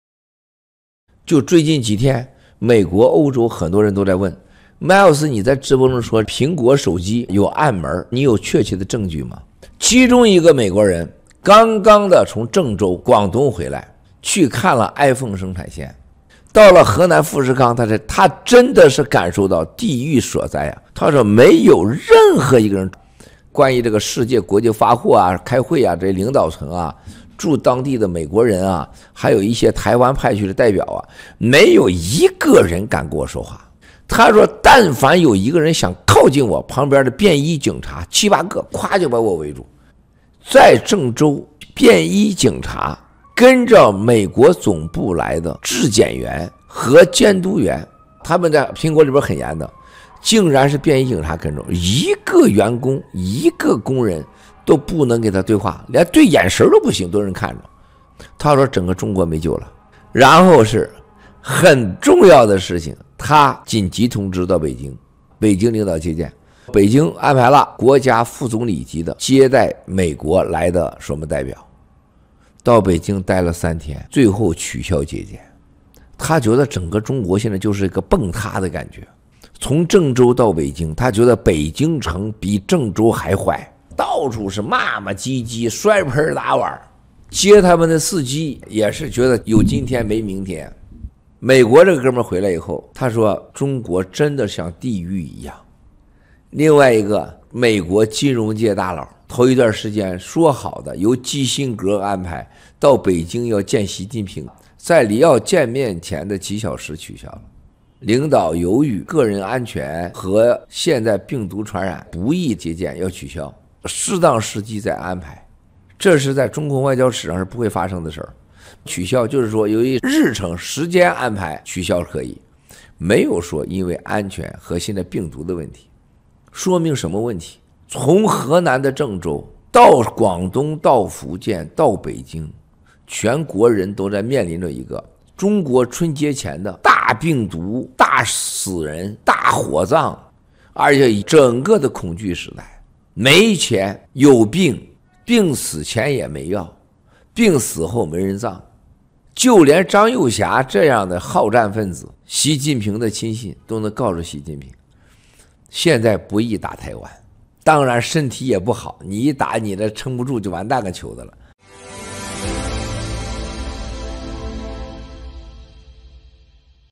2023.01.19 Recently, an American returned from the iPhone production line in China.
He said no one dared to talk to him when he was there. The plainclothes police officers would surround him whenever people tried to approach him, and even when they made eye contact, they were watched. A US financial bigwig had been arranged by Kissinger to meet Xi Jinping in Beijing. Just a few hours before the meeting, he was told that the meeting was canceled due to concerns over Xi's personal safety and the current covid outbreak. Right before Chinese New Year, Chinese people are experiencing covid outbreaks, excessive deaths, and funerals. This is a period of trepidation.
最近几天一个美国人刚刚的从苹果手机生产线回来，没有一个人敢跟他说话，但凡有一个人想靠近他旁边的便衣警察就把他围住，连对眼神不行，都有人看着。一个美国金融界大佬，由基辛格安排到北京要见习近平。